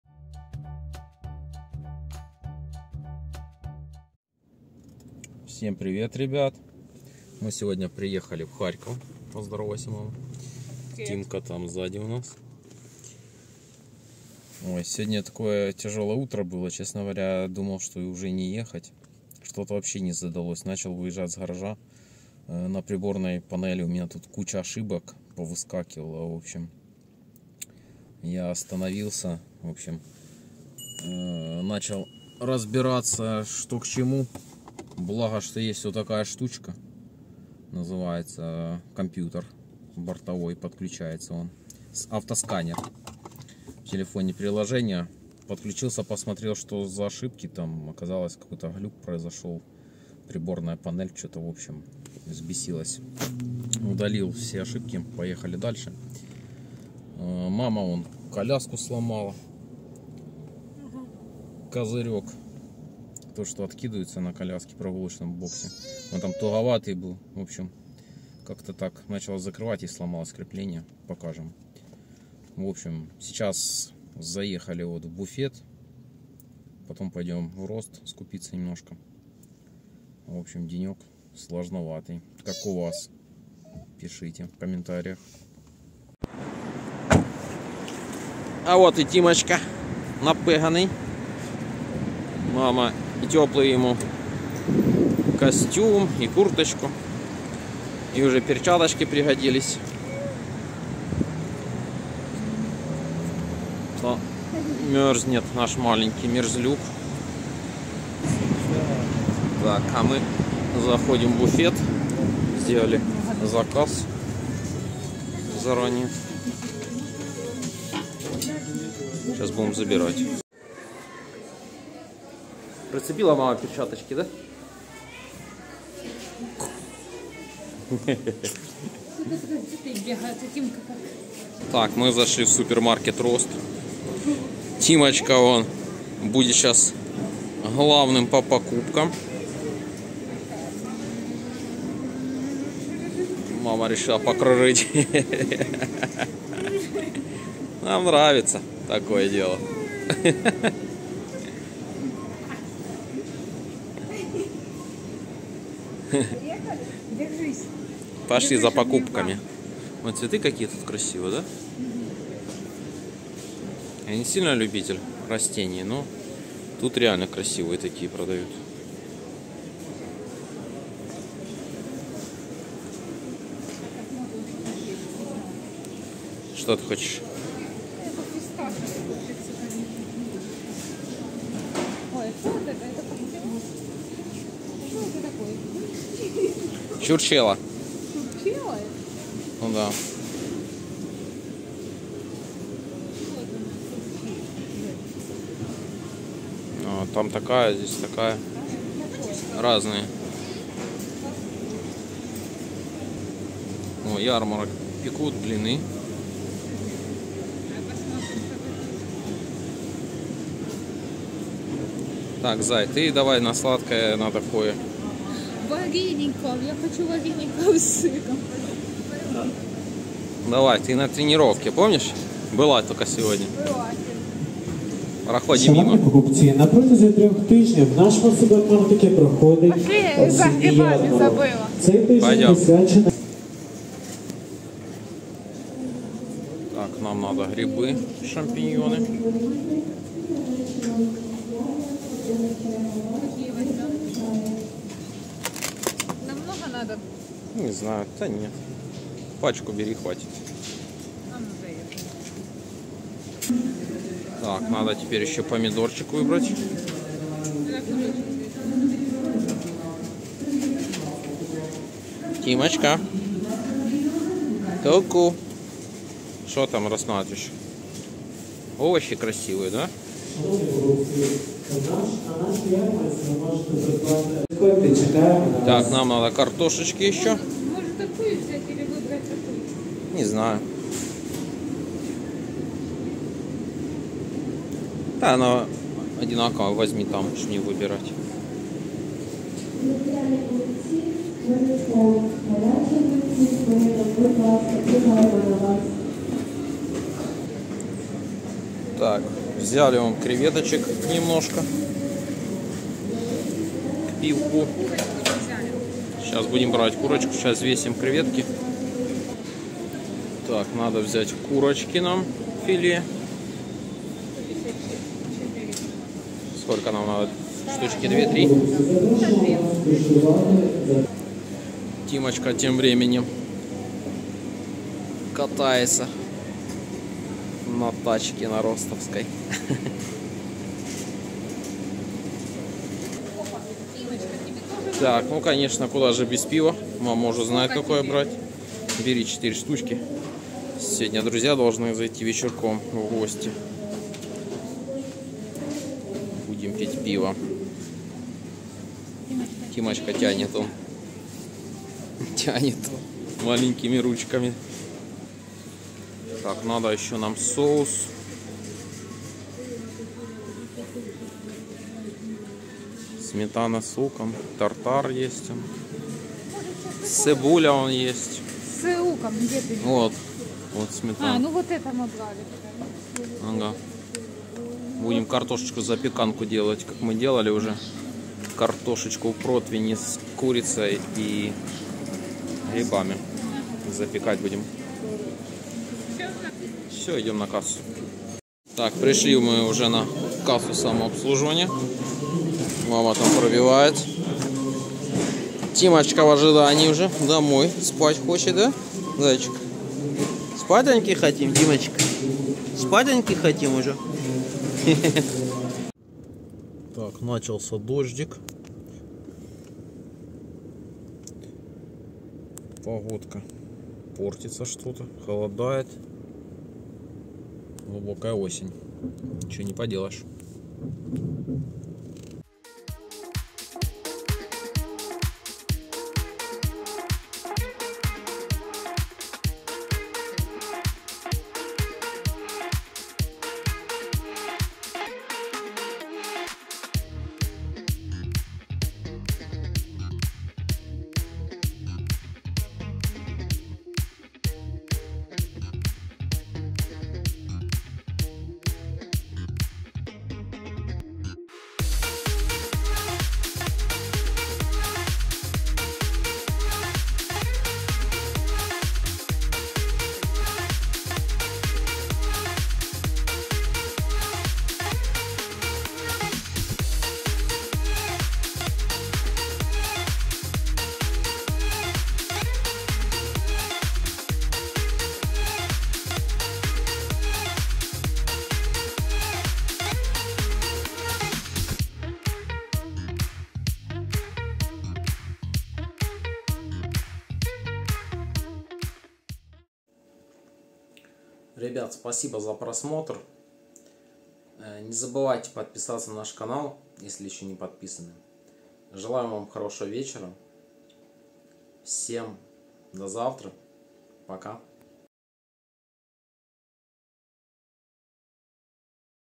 VLOG📌 РЕВИЗОР ПОКУПОК🧤 ВЗЯЛИ ТАЧКУ НА ПРОКАТ ТИМУ В РОСТЕ 🛒СНИМАЕМ ТИК ТОК С ДРУЗЬЯМИ🎦
Всем привет!👋Добро пожаловать в наш новый VLOG, в этом видео мы поехали в Рост за покупками🛒, скупиться на вечер, так как ожидаем у себя гостей.🍻 Конечно же скупится без ревизора нам не удалось 😄Вечером с друзьями после застолья, решили снять Тик Ток все вместе.🎉 Надеемся вам понравится это видео. Приятного вам просмотра!🍿

Временные эпизоды:

00:00 - Введение
00:08 - Приветствие на канале
00:18 - Тяжелое утро
01:30 - Идём в буфет за пицой
02:12 - Зашли в супермаркет Рост за продуктами
06:46 - Едем домой
07:03 - Снимаем Тик Ток с друзьями
08:11 - Окончание видео
08:26 - Концовка